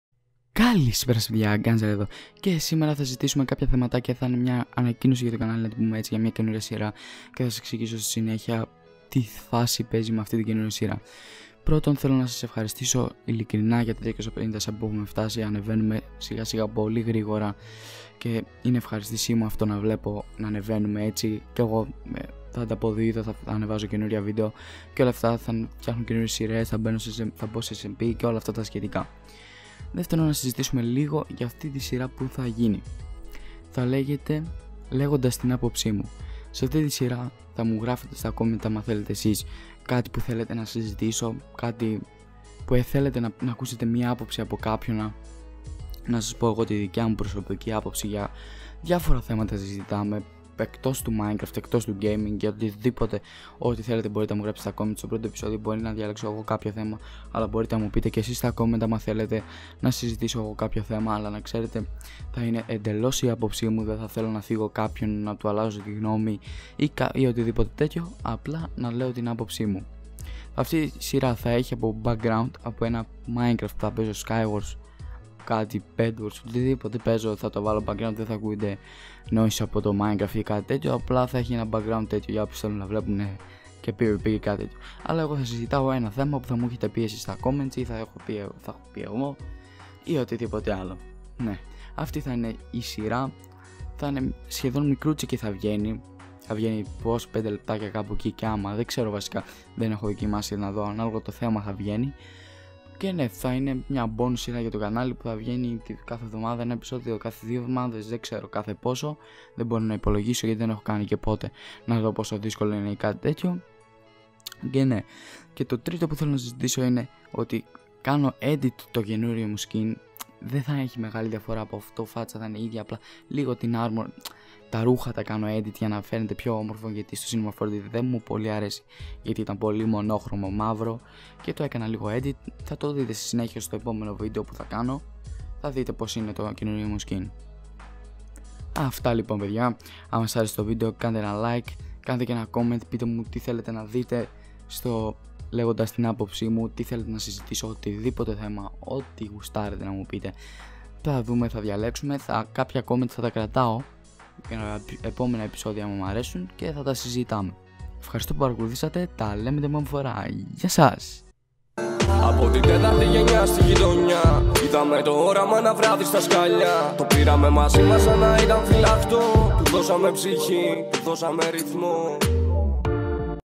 ΛΕΓΟΝΤΑΣ ΤΗΝ ΑΠΟΨΗ ΜΟΥ (Νέα σειρά ανακοινώσεις)
Πείτε μου κάτω στα Comments τι θα θέλατε να συζητήσω στο επεισόδιο 1 (Ότι θέμα θέλετε και έκτος minecraft)

IGN : _Gunzar_
Discord Name : Gunzar#4154
Άμα κάνατε ένα Share θα το εκτιμούσα
● 1 Like αμα σας άρεσε το Βιντεο

Ευχαριστώ που παρακολουθήσατε